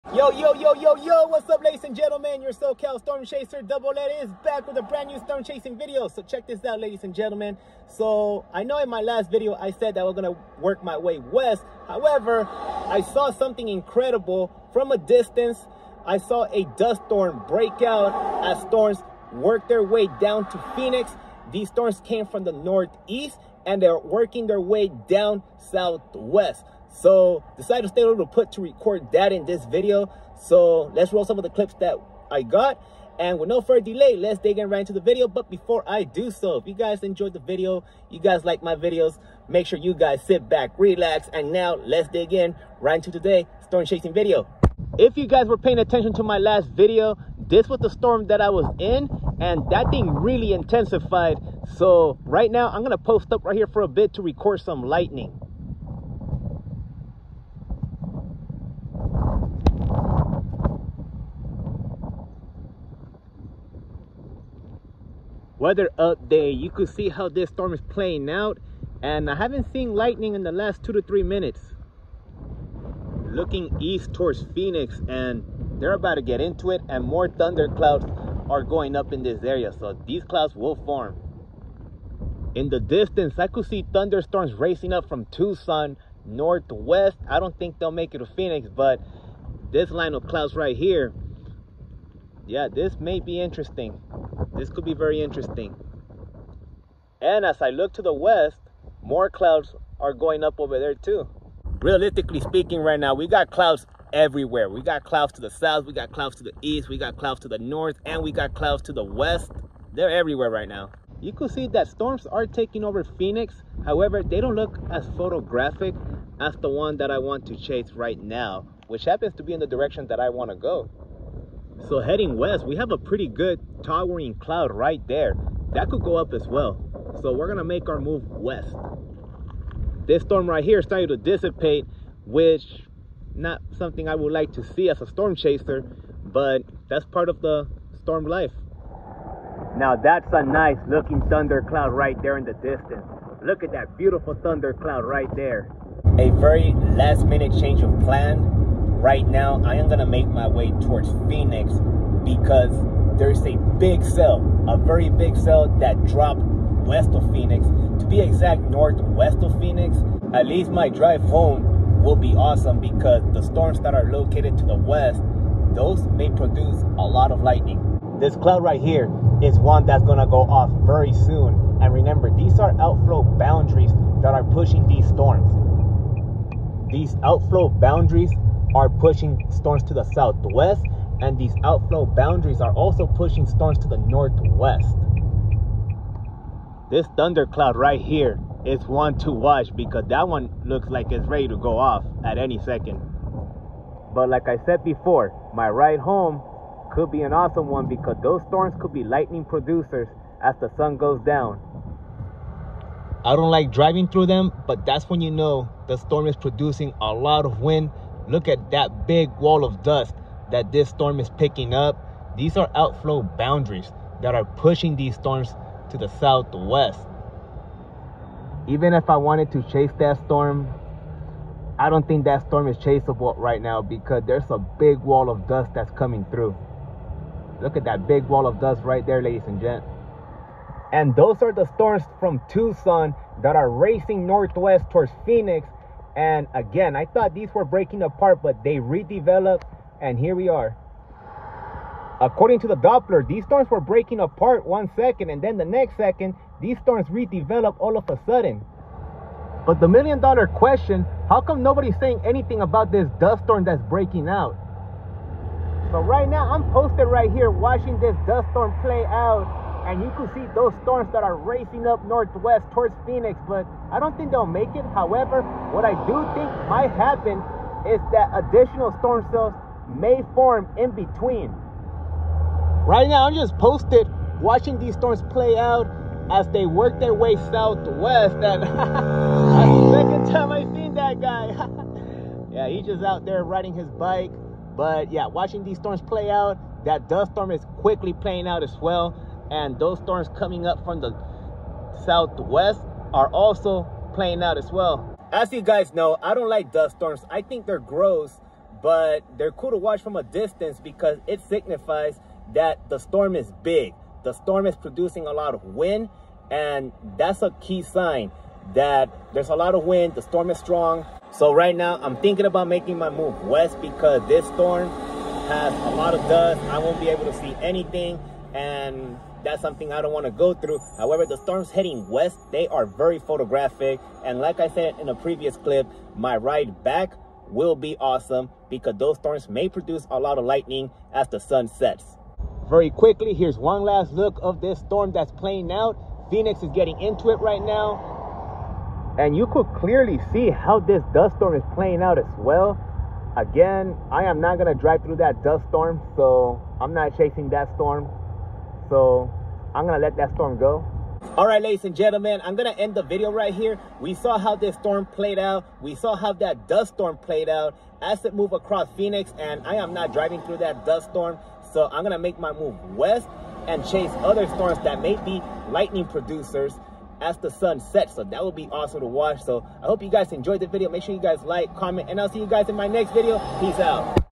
Chasing the Monsoon ⛈️ Thunderstorm and Dust Storm west of Phoenix 💨
A complex of severe thunderstorm northwest of Phoenix produced a large dust storm !